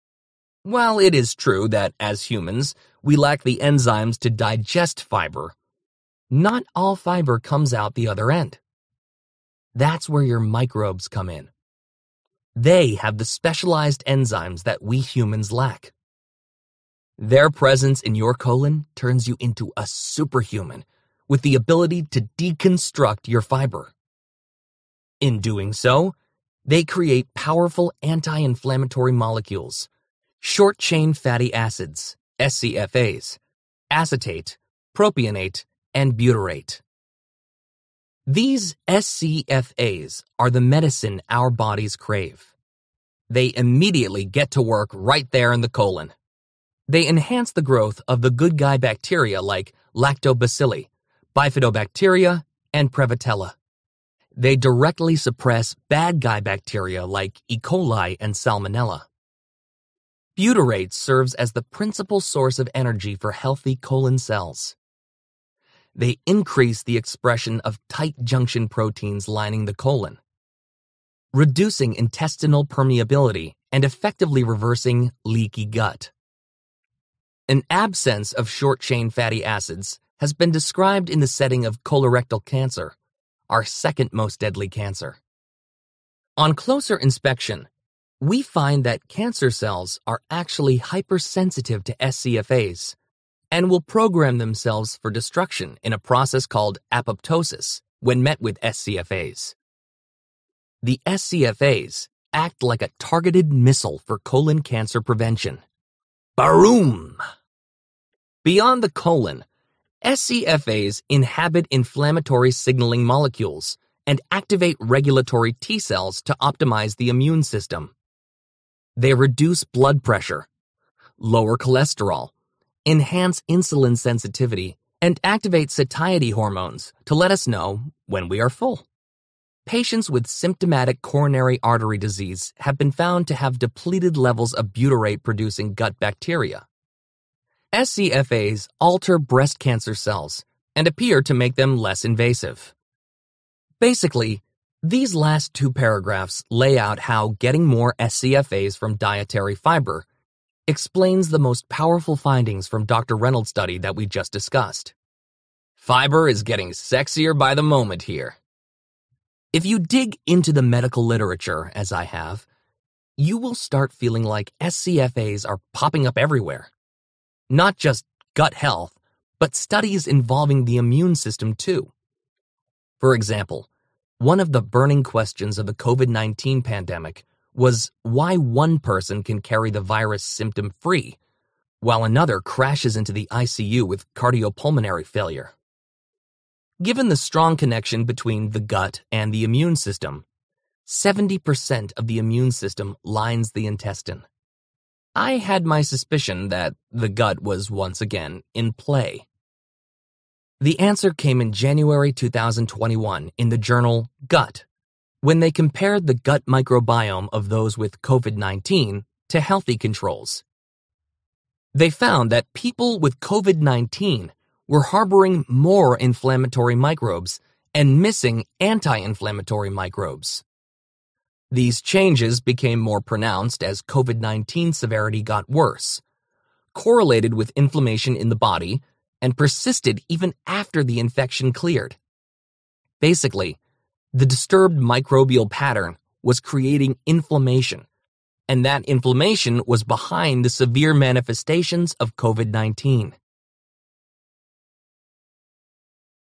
The Fiber Fueled By Will Bulsiewicz
Leading gastroenterologist Dr. Will Bulsiewicz, or “Dr. B,” introduced readers to the wonders of fiber with the New York Times bestseller Fiber Fueled—a guide to optimizing the gut microbiome, sharpening immunity, lowering cholesterol, and promoting weight loss through a diet rich in diverse fruits, vegetables, nuts, seeds, and legumes. Rather than restriction, Dr. B’s solution is abundance and variety.